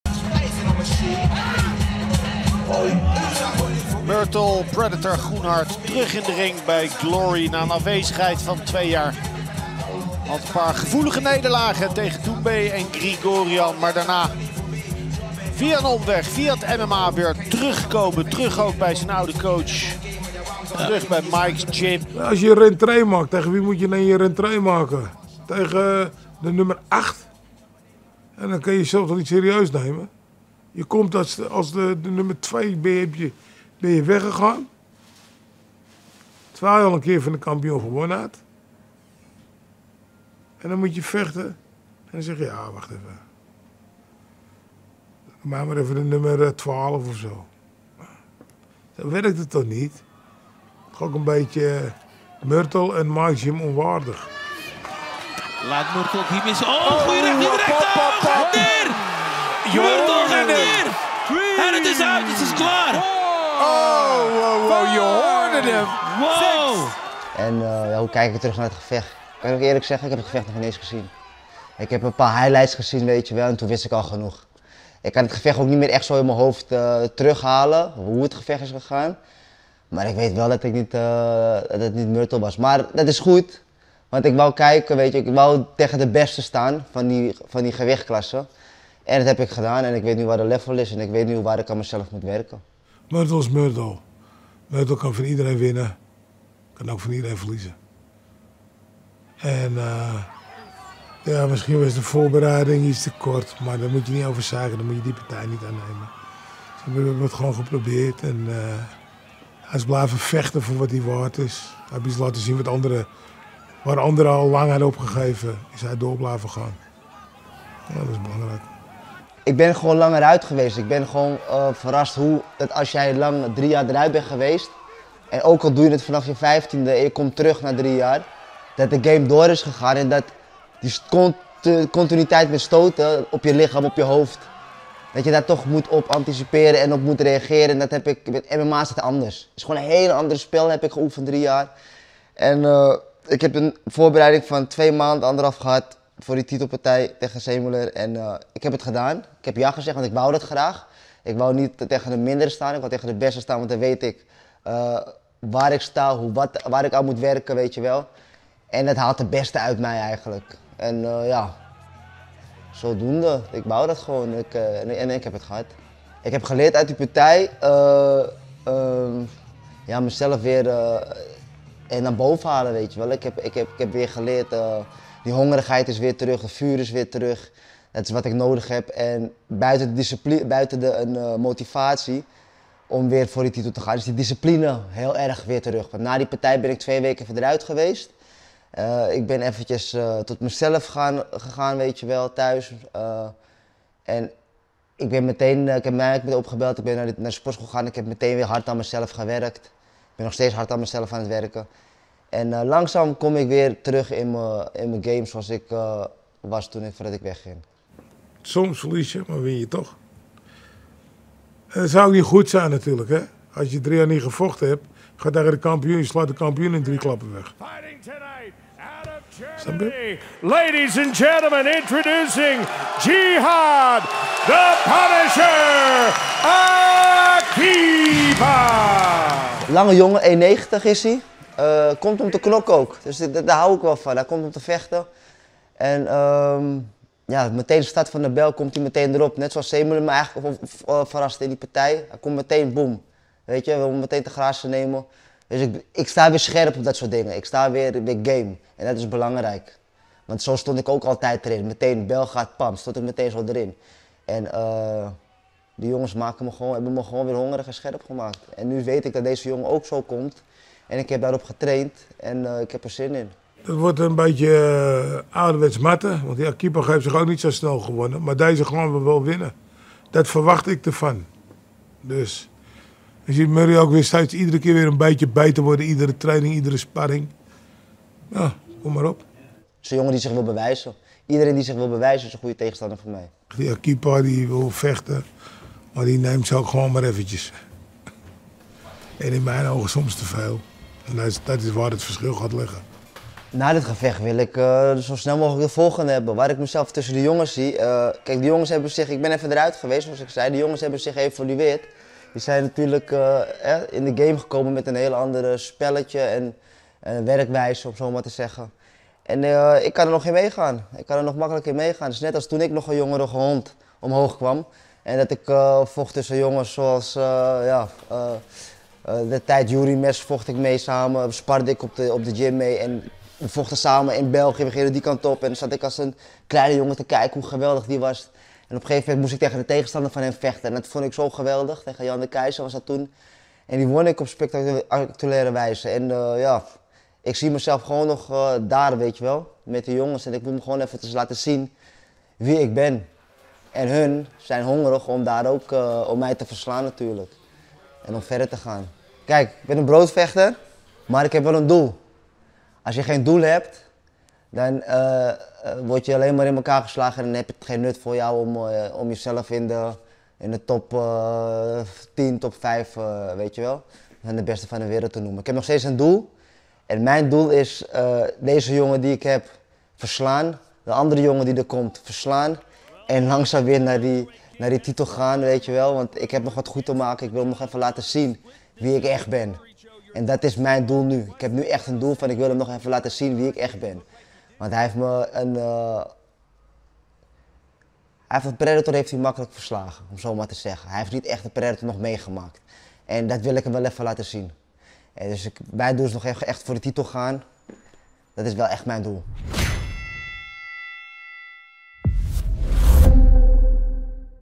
Murthel Groenhart: “Ik wil laten zien wie ik echt ben, de titel is m’n doel” | Glory 87
Glory 87 kijk je zaterdag 19 augustus live bij Videoland vanaf 19:30! Met op de fight card:

Beztati vs. Kaito
Groenhart vs. Akipa
Boapeah vs. Braun
Kaffa vs. El Hammouti
Jürjendal vs. Terpstra
Rajabzadeh vs. Amine